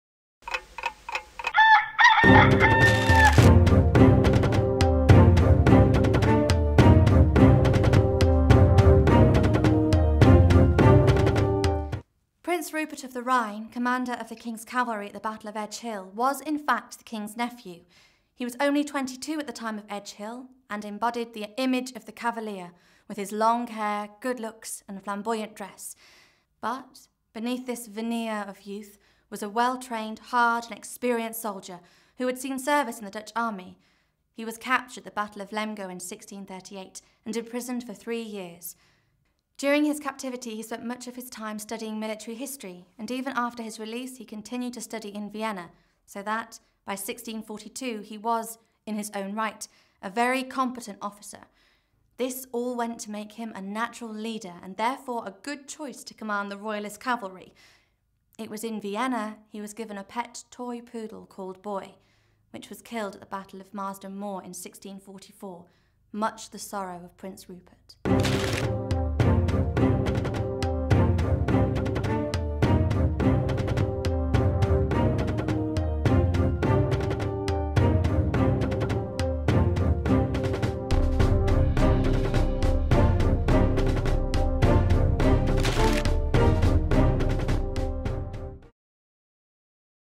The English Civil War | Prince Rupert | KS3 History
This short video describes the flamboyant cavalier Prince Rupert, who was not only commander of the King's cavalry but also his nephew.
With core content of 60 seconds, this video has keywords throughout to support literacy and a quick summary of key points to recap at the end.
History for kids, KS3 History, KS3, English Civil War, the Civil War, 17th Century,